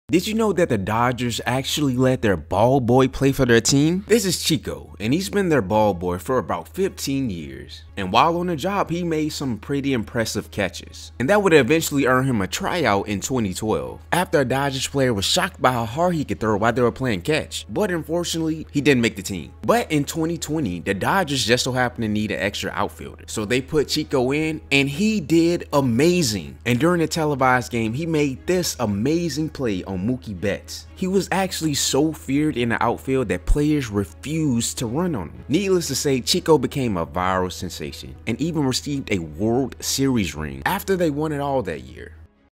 The Dodgers Let Their Ball Boy Play In A Game | RevUpSports.com
The Los Angeles Dodgers gave their ball boy a once-in-a-lifetime opportunity to step onto the field and play in a real game! Watch as he makes his debut and shows off his skills in front of a packed stadium. This heartwarming moment is sure to inspire and uplift any sports fan! 🏀💪🎉

Gear Up For *YOUR* Game At RevUpSports.com